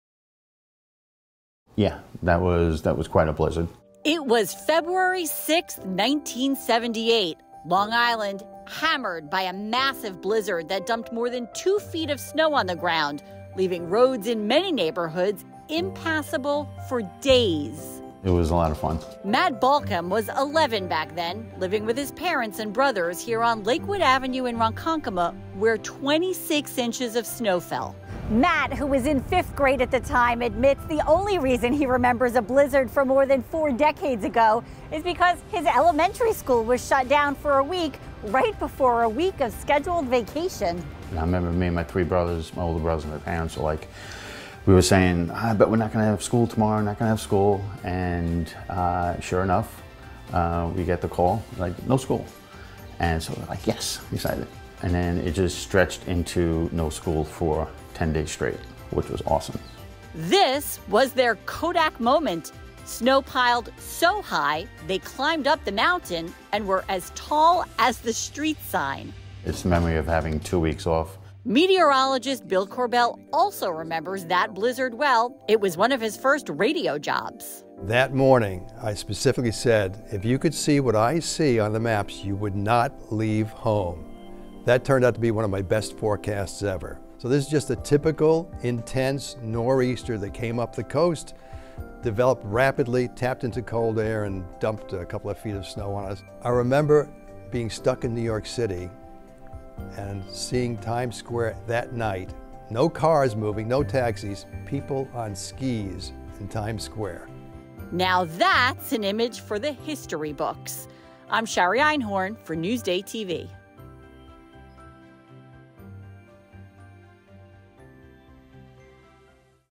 Monster Storms: Blizzard of 1978
Long Island was paralyzed when more than 2 feet of snow fell. It took days to dig out.